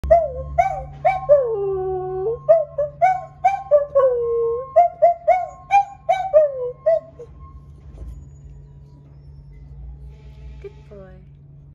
Sometimes my dog Dumpling tries to howl like a wolf! 🐾🐶👂📢
When Dumpling hears the trash truck beeping, he howls is response! 🥰🐾🐶👂.